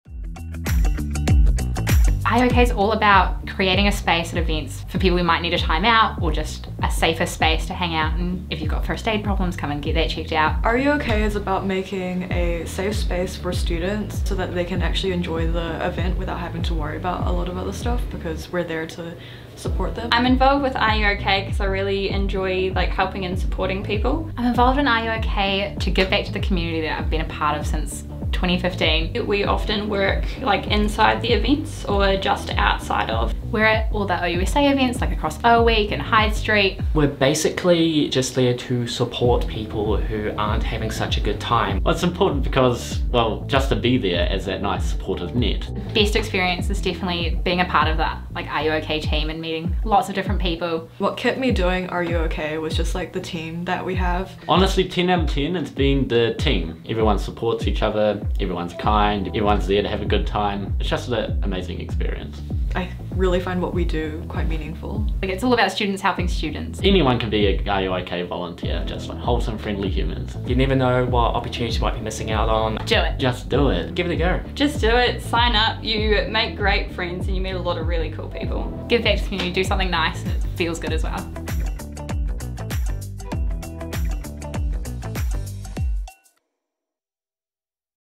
ARE YOU OKAY
Are You OK? is the OUSA Student Support safety team, here to make sure that a good time doesn’t turn into a bad time and support is always there when it is needed. The Are You OK? safety team provides students with a non-judgmental, approachable, and friendly source of support.